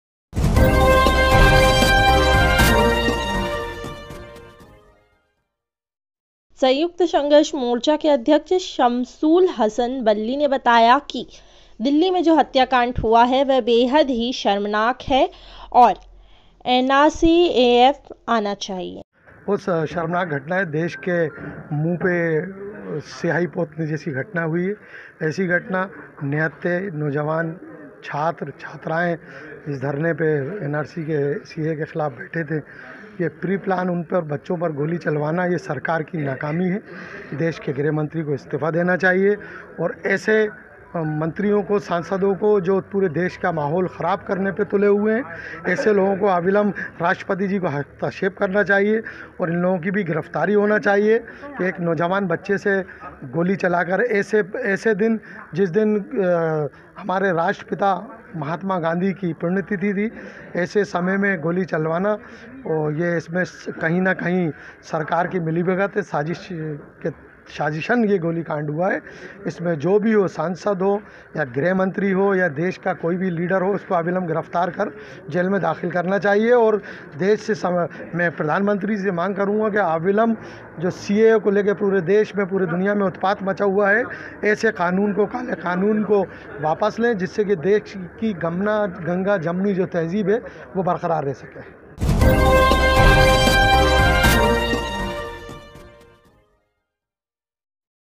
दिल्ली का गोलीकांड बेहद शर्मनाक।
संयुक्त संघर्ष मोर्चा के  शमसुल हसन ने बताया कि दिल्ली में जो गोलीकांड हुआ है वह शर्मनाक है